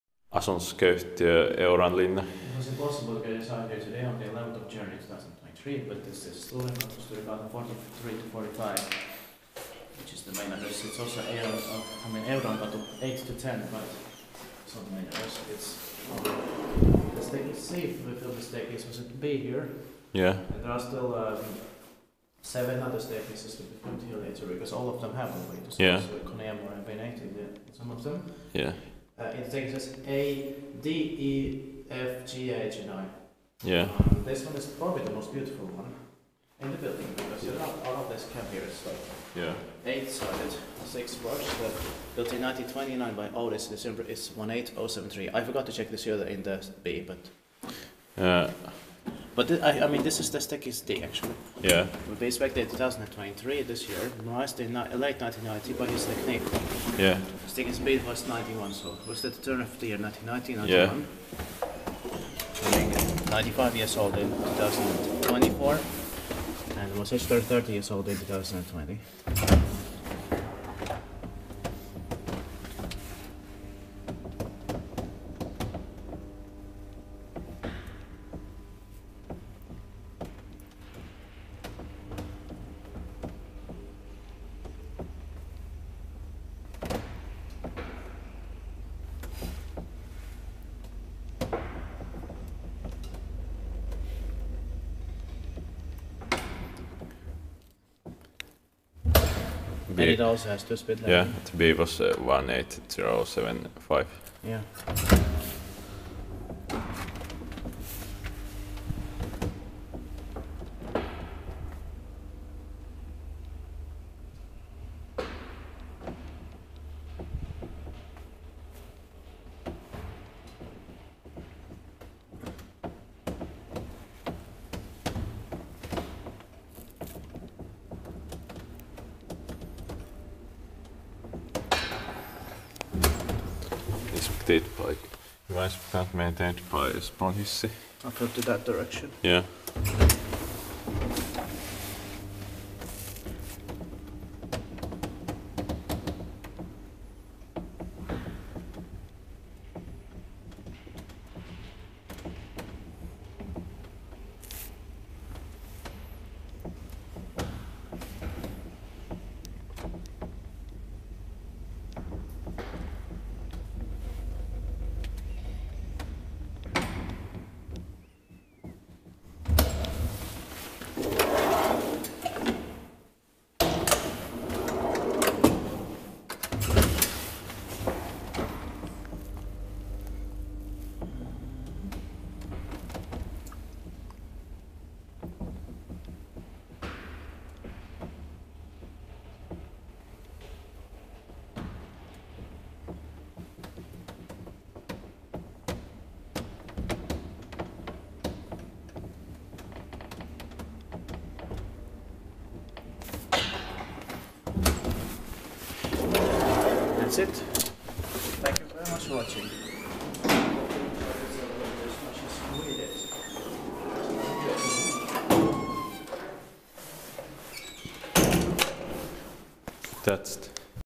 Old Otis (mb Hissitekniikka) traction elevator/lift in Sturenkatu 43-45D, Vallila, Helsinki
Location: Sturenkatu 43-45D, Vallila, Helsinki, Finland
Brand: Otis/Hissitekniikka, Filmed with Elevators from Finland
Year installed/modernized: 1929/1991
Serial number: 18073
Floors: 5(1, 2, 3, 4, 5)
Capacity: 3 persons or 240 kg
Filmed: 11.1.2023